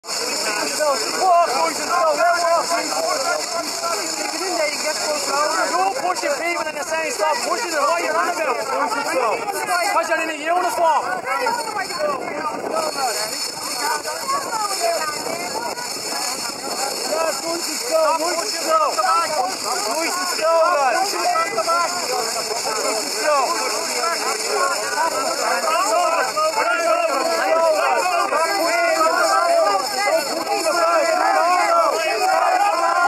Gardai trying to incite crowd after RTE cameras have left
RTE turns up with a camera and the majority of Gardai disappear and the workers go for lunch. The camera leaves and they all reappear and straight away the gardai move in and start pushing into all those who have been standing peacefully protesting against the installation of water meters